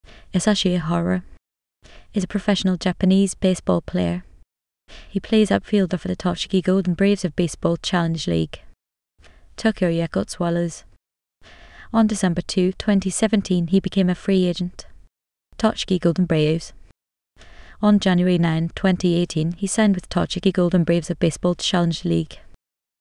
Yasushi Iihara
is a professional Japanese baseball player. He plays outfielder for the Tochigi Golden Braves of Baseball Challenge League.